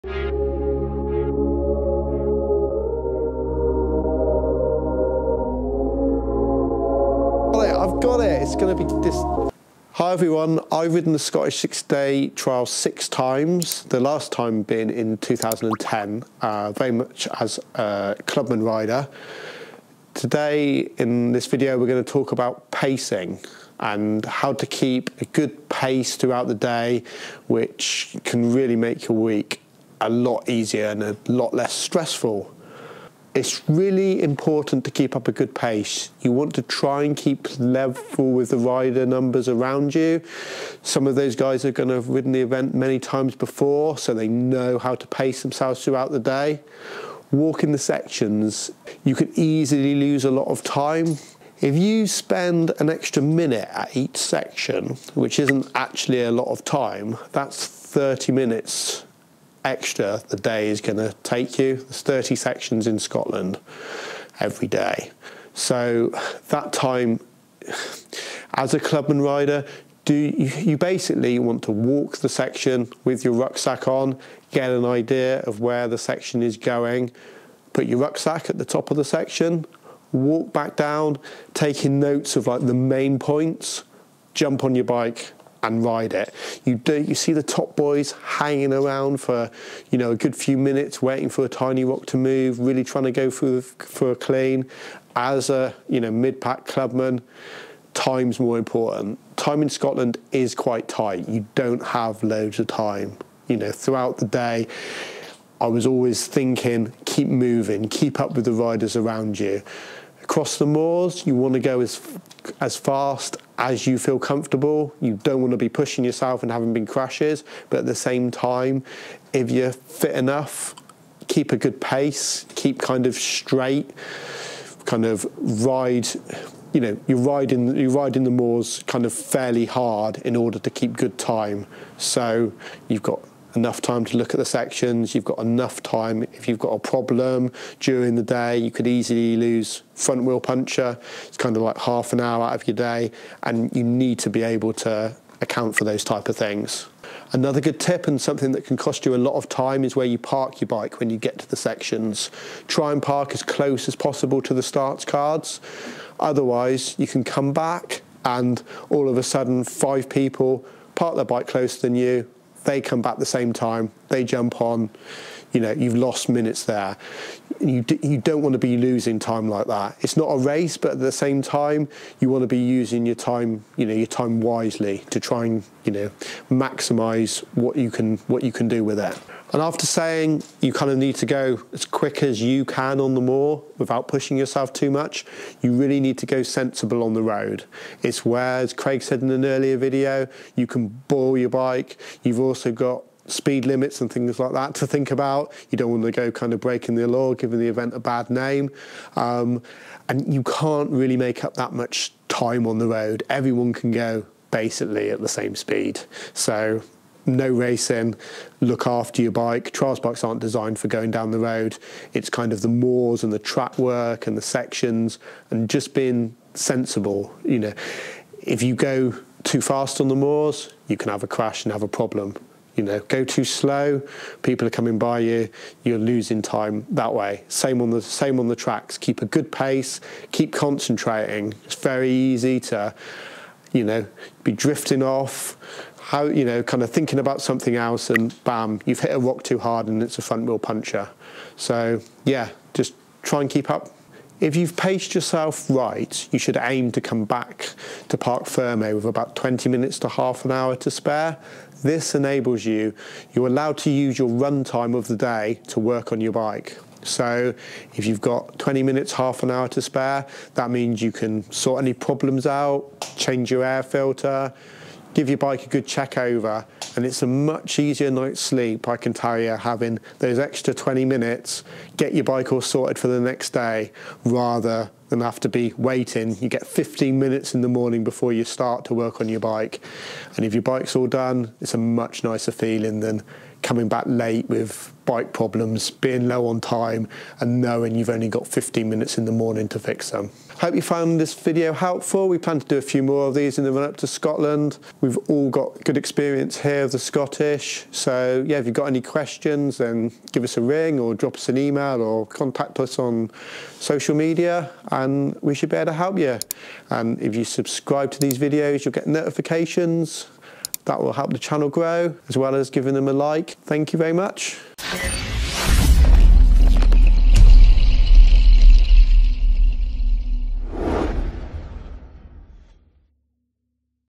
SSDT PREP - Time Management
Not long till the Scottish Six Day Trial Event! We are making a couple of helpful tip videos for those competing, enjoy!